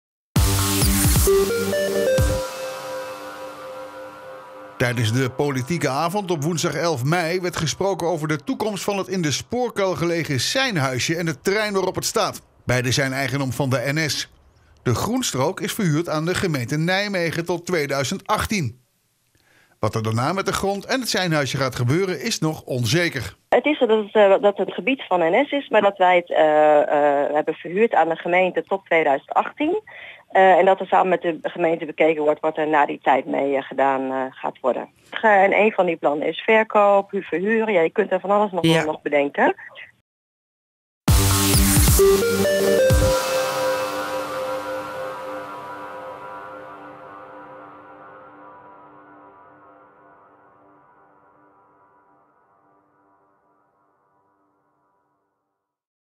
Toekomst seinhuisje vooralsnog onzeker
Tijdens de Politieke Avond op woensdagavond 11 mei werd gesproken over de toekomst van het in de Spoorkuil gelegen seinhuisje en het terrein waarop het staat. Beide zijn eigendom van de NS.